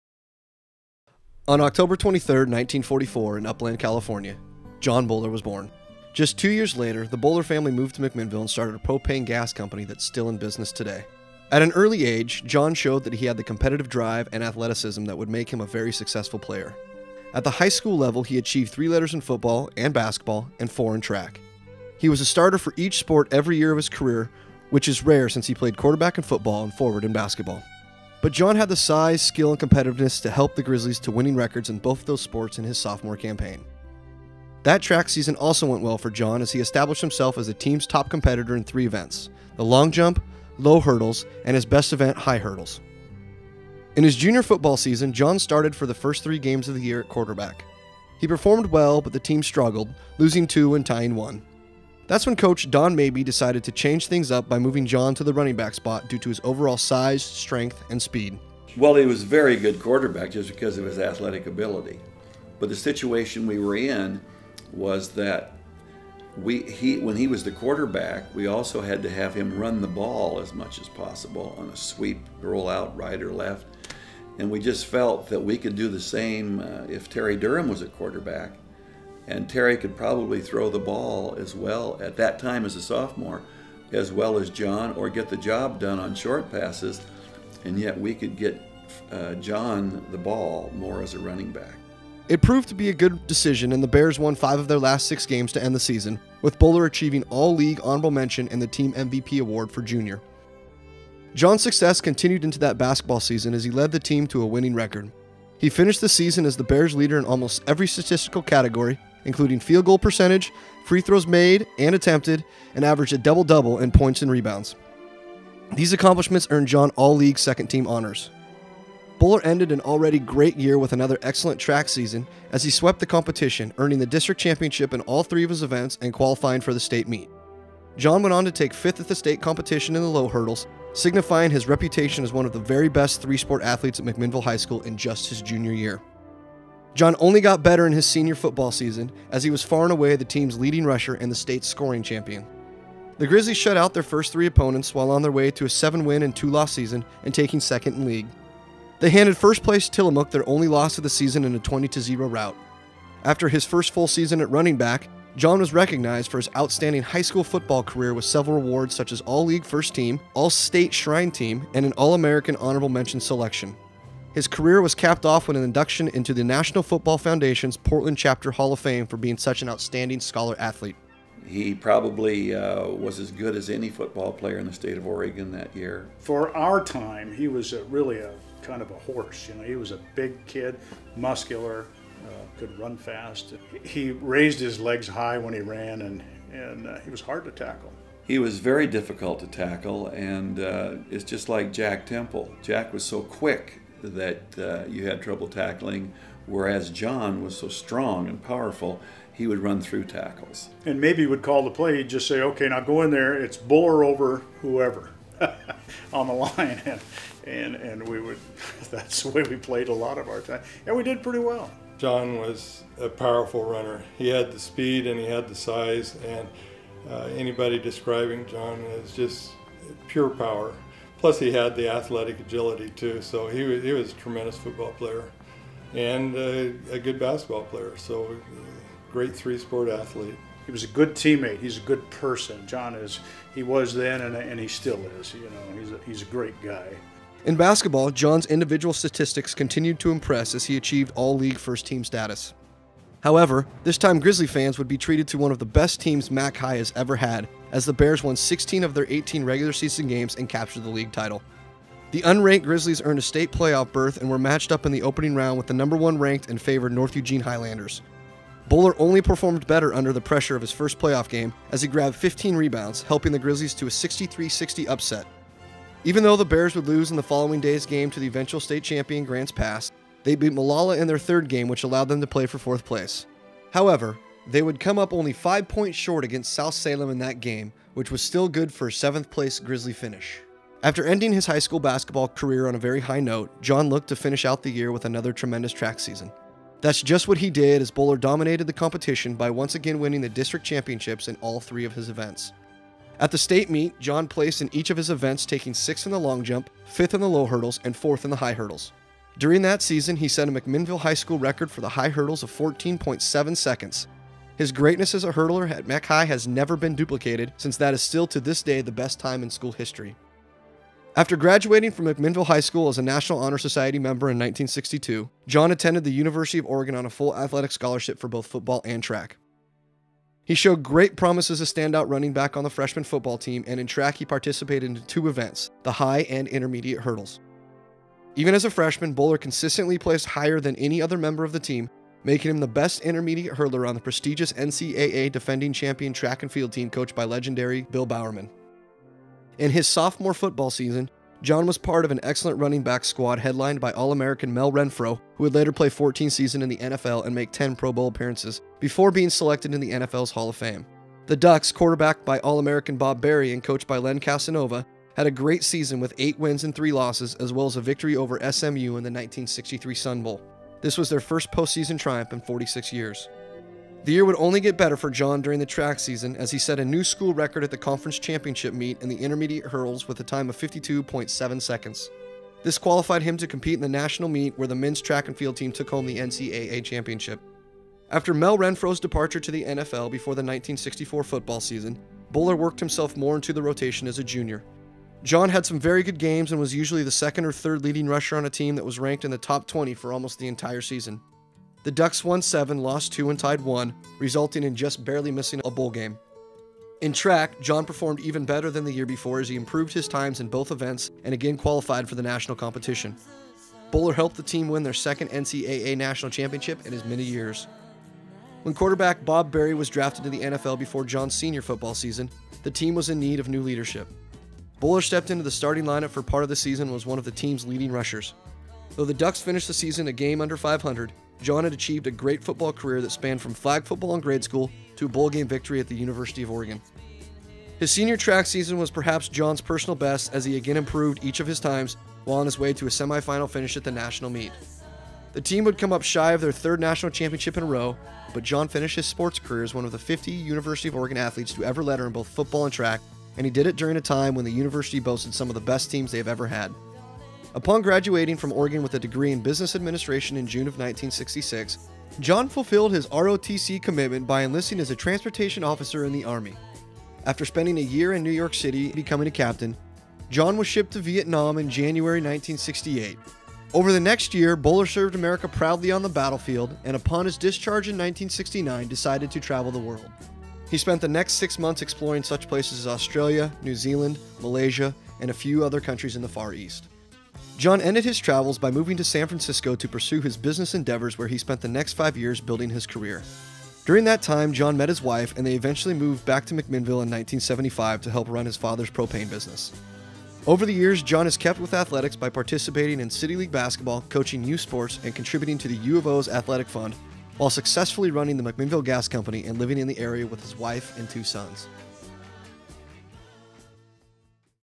John Buller Documentary
2008 MHS Sports Hall of Fame Induction Video of 3 sport high school standout and 2 sport college athlete, John Buller.